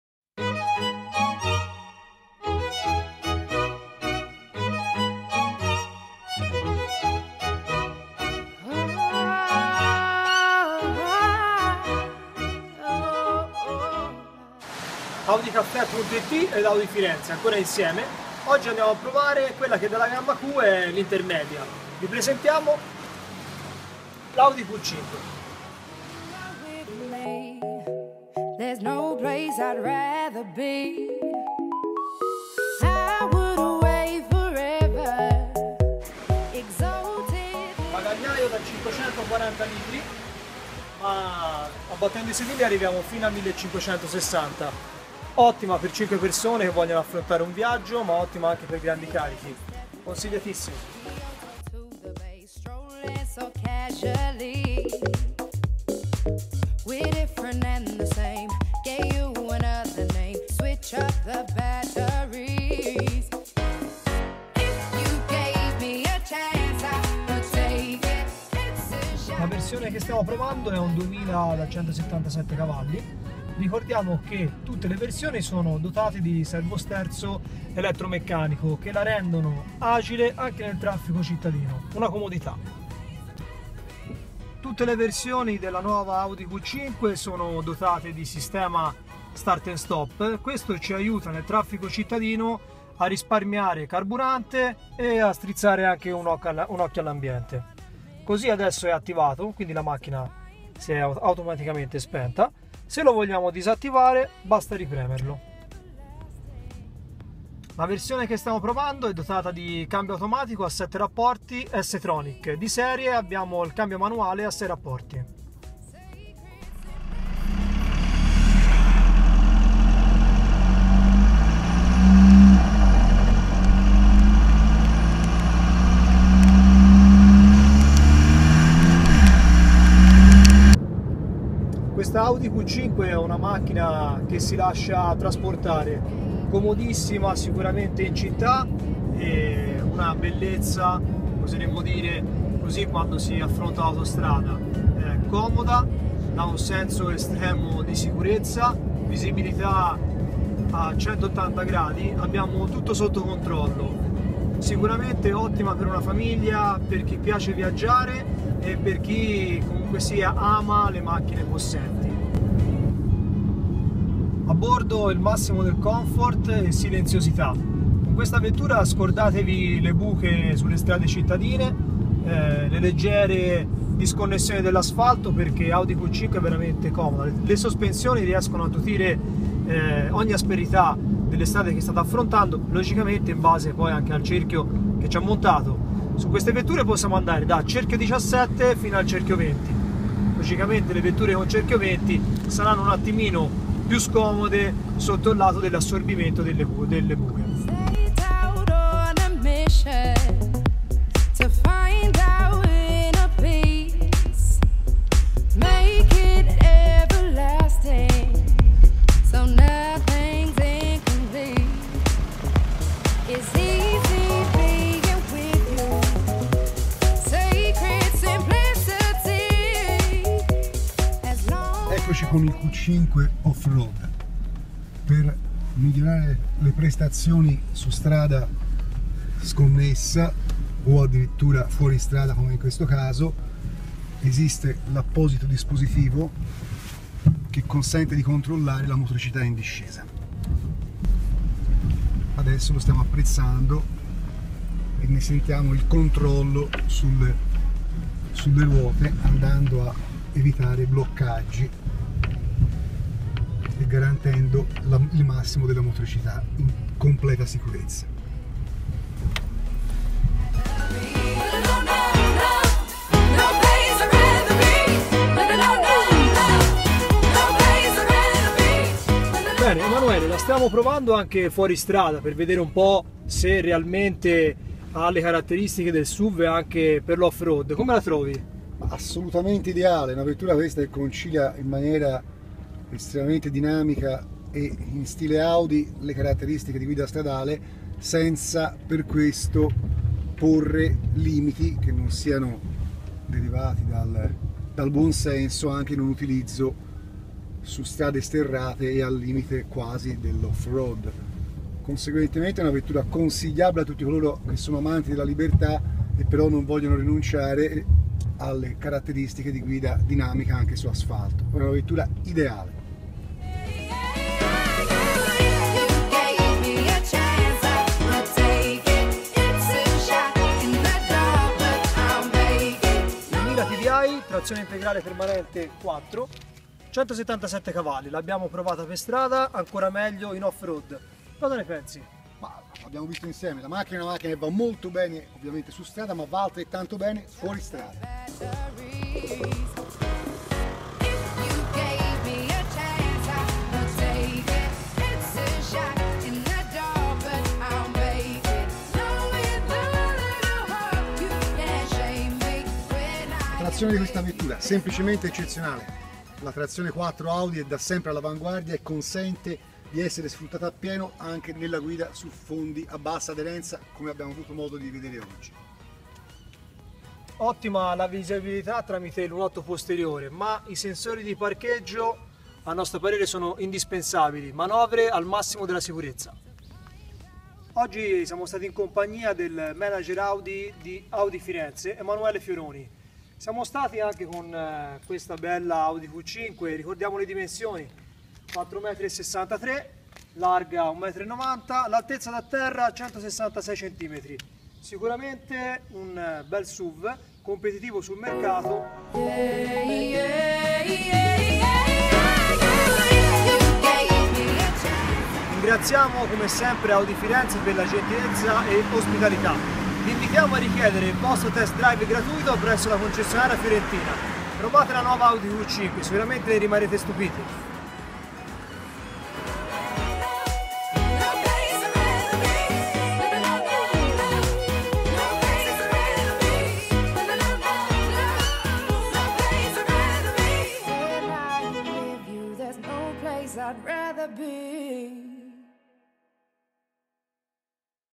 Test drive Audi Q5 2.0 TDI 177 cv S-Tronic - Audicafe.it
Abbiamo avuto la possibilità di provare la nuova Audi Q5 in versione 2.0 TDI 177 cv e cambio automatico a 7 rapporti S-Tronic, in questo video vi racconteremo qualche curiosità oltre i soliti dati tecnici e vedrete come il Q5 se la cavi bene per un utilizzo a 360°, dall'asfalto cittadino all'offroad  impegnativo.